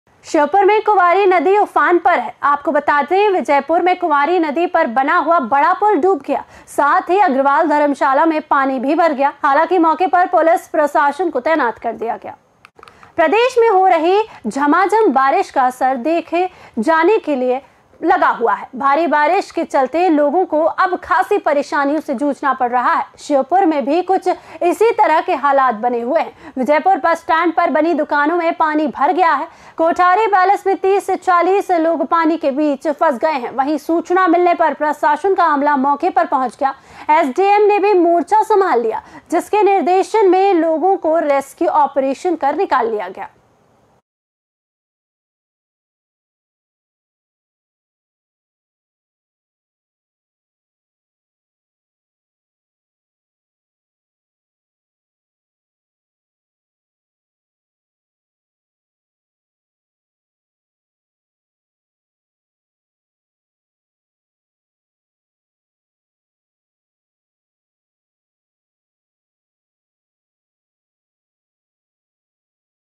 उफान पर कुंवारी नदी, अग्रवाल धर्मशाला में भरा पानी!
उफान पर कुंवारी नदी,
अग्रवाल धर्मशाला में भरा पानी!

BREAKING NEWS